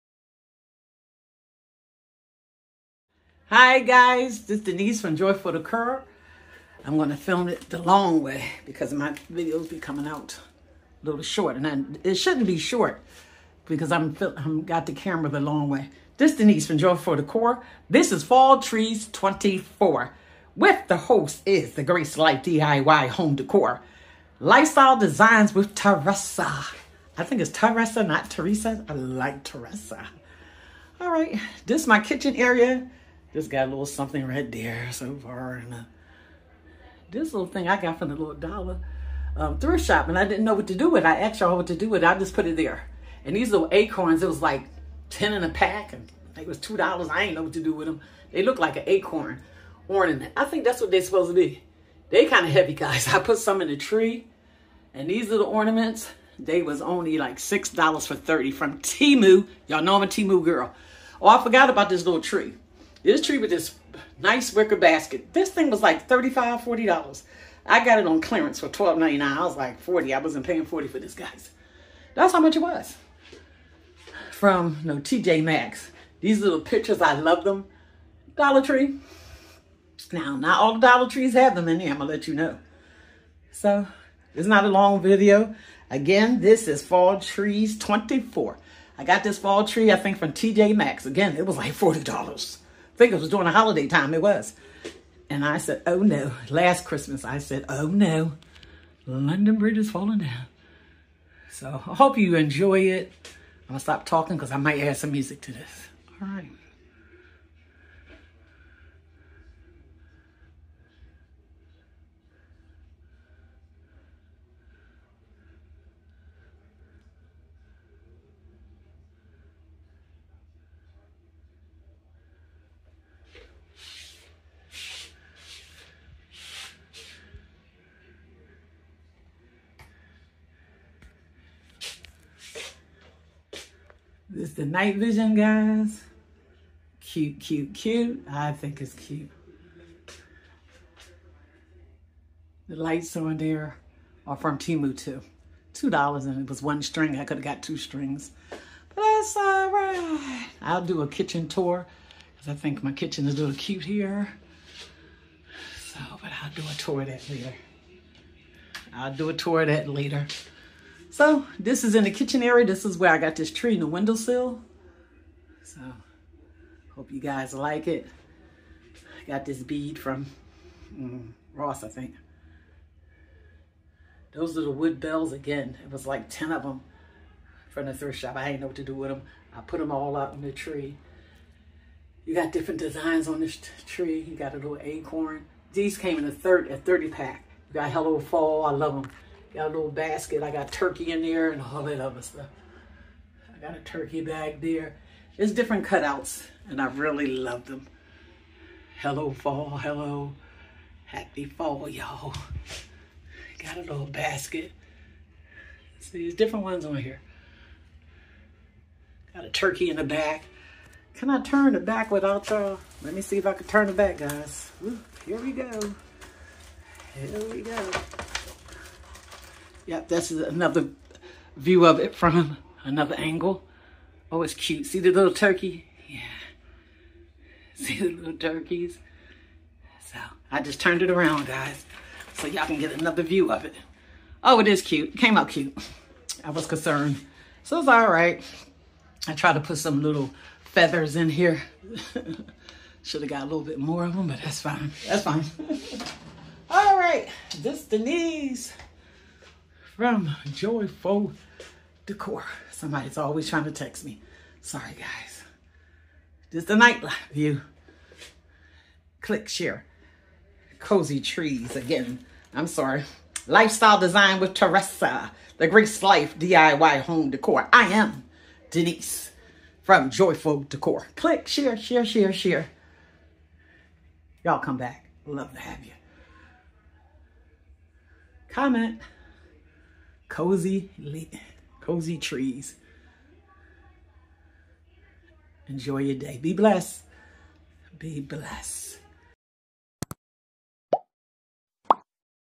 FallTrees24 # JoyFull Decor By Denise
Come with me has we design a  tree big tree small tree, for Fall your Way Your Style

The colors are vibrant seasons are changing what are you waiting for Explor Create have Fun

Instragram.Virlee01